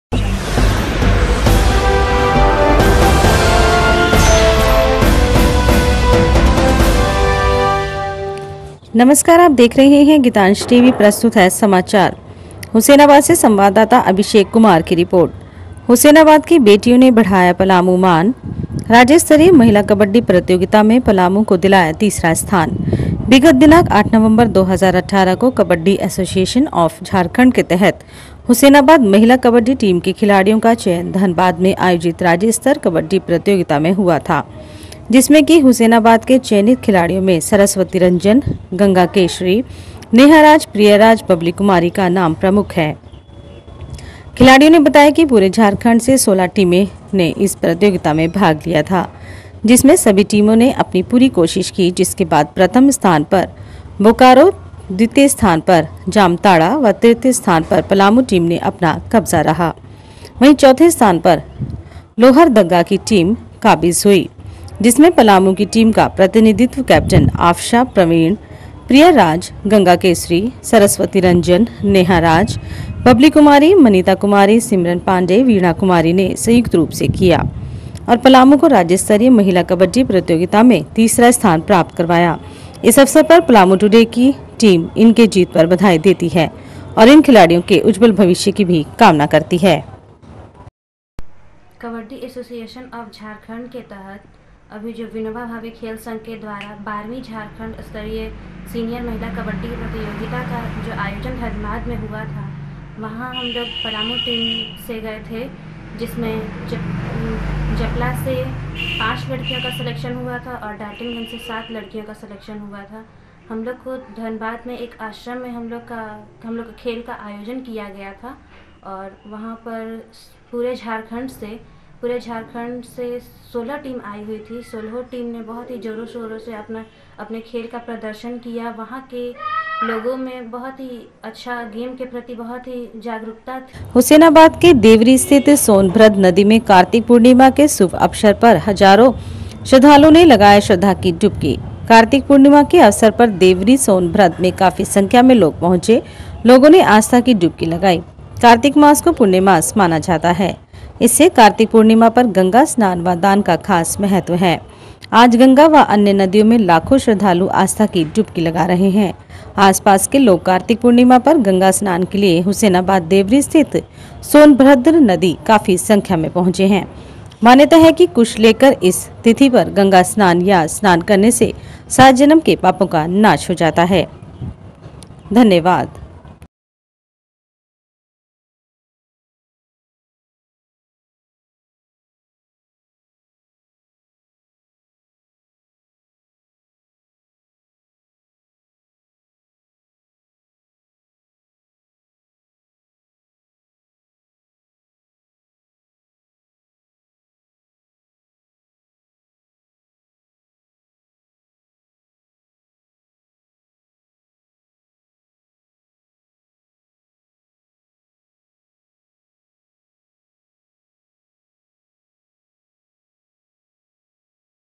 बेटियों ने बढ़ाया मान,राज्य स्तरीय महिला कब्बडी प्रत्तियोगिता मे पलामू को दिलाया तीसरा स्थान
हुसैनाबाद की बेटियों ने बढ़ाया पलामू मान,राज्य स्तरीय महिला कब्बडी प्रत्तियोगिता मे पलामू को दिलाया तीसरा स्थान
बिगत दिनांक 08 नवम्बर.2018.को कब्बड्डी अस्सोसिएसन ऑफ़ झारखण्ड के तहत हुसैनाबाद महिला कबड्डी टीम के खिलाड़ियों का चयन धनबाद में आयोजित राज्य स्तरीय कबड्डी प्रतियोगिता हुआ था जिसमे की हुसैनाबाद के चयनित खिलाड़ियों में सरस्वती रंजन,गंगा केशरी,नेहा राज, प्रिया राज , बबली कुमारी, का नाम प्रमुख हैं
खिलाड़ियों ने बताया कि पुरे झारखण्ड से 16 टीमें ने इस प्रत्तियोगिता में भाग लिया था
जिसमे सभी टीमो ने अपनी पूरी कोशिश की जिसके बाद प्रथम स्थान पर बोकारो द्वितीय स्थान पर जामताड़ा व तृतीय स्थान पर पलामू टीम ने अपना कब्ज़ा रहा वही चौथे स्थान पर लोहरदग्गा की टीम काबिज़ हुई
जिसमे पलामू की टीम का प्रतिनिधित्व कैप्टन आफ्शा प्रवीण,प्रिया राज,गंगा केशरी,सरस्वती रंजन,नेहा राज,बबली कुमारी,मनिता कुमारी,सिमरन पाण्डेय,विणा कुमारी ने संयुक्त रूप से किया और पलामू को राज्य स्तरीय महिला कब्बडी प्रत्तियोगिता मे तीसरा स्थान प्राप्त करवाया
इस अवसर पर पलामू टुडे की टीम इनके जीत पर बधाई देती है और इन खिलाड़ियों के उज्जवल भविष्य की कामना करती है
हुस्सैनाबाद के देवरी स्थित सोनभद्र नदी में कार्तिक पूर्णिमा के सुभ अवसर पर हजारों श्रद्धालुओ ने लगाया श्रद्धा की डुबकी।
कार्तिक पूर्णिमा के अवसर पर देवरी सोनभद्र में काफी संख्या में लोग पोहचे लोगो ने आस्था की डुबकी लगाई,कार्तिक मास को पुण्य मास माना जाता है। इससे कार्तिक पूर्णिमा पर गंगा स्नान व दान का खास महत्व है। आज गंगा व अन्य नदियों में लाखों श्रद्धालु आस्था की डुबकी लगा रहे है। आसपास के लोग कार्तिक पूर्णिमा पर गंगा स्नान के लिये हुसैनाबाद देवरी स्थित सोनभद्र नदी काफी संख्या में पोहचे हैं।
मान्यता है कि कुश लेकर इस तिथि पर गंगास्नान या स्नान करने से सात जन्म के पापों का नाश हो जाता है।